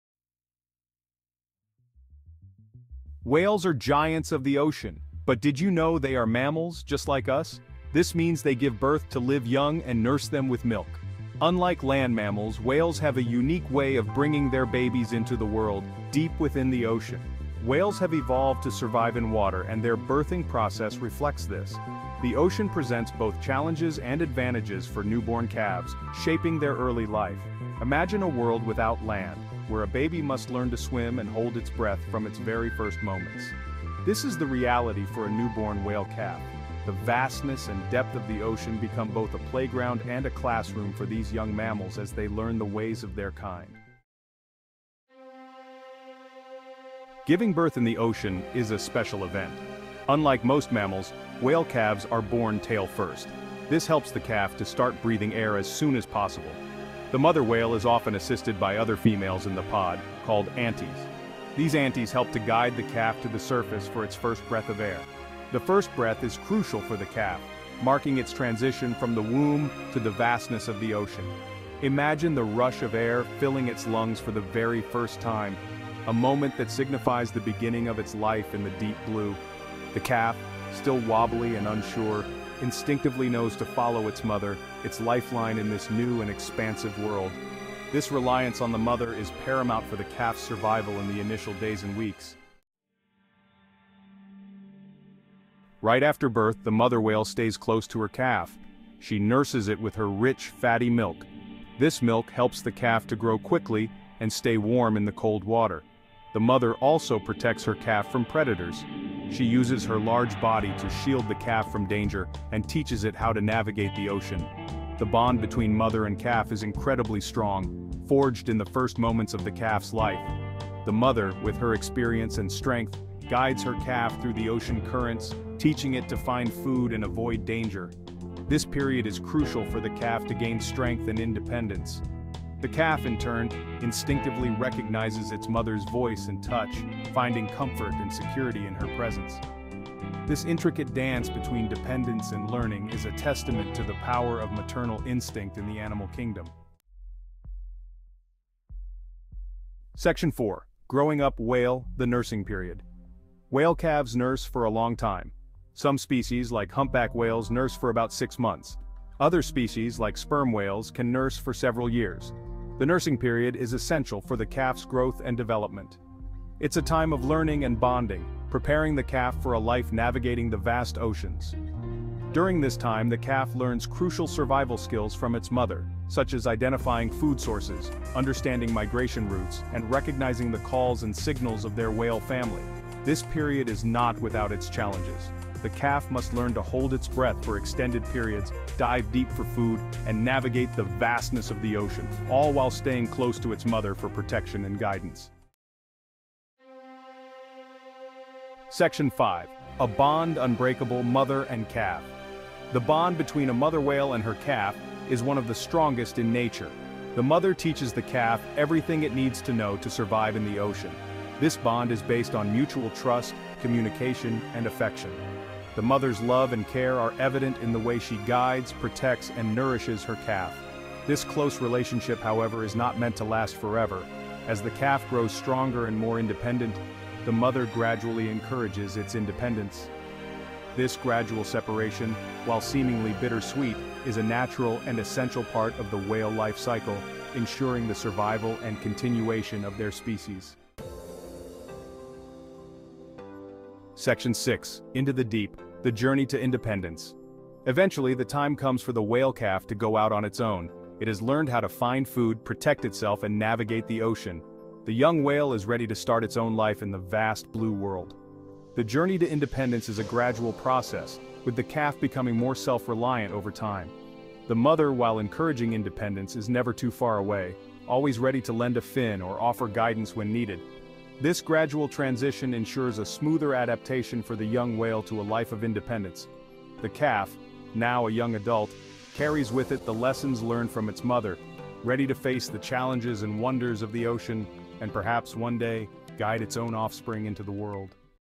How do Whales give Birth and Nurse?
"Whale Miracles: Discovering the Fascinating Birth and Nursing Process in the Depths of the Ocean"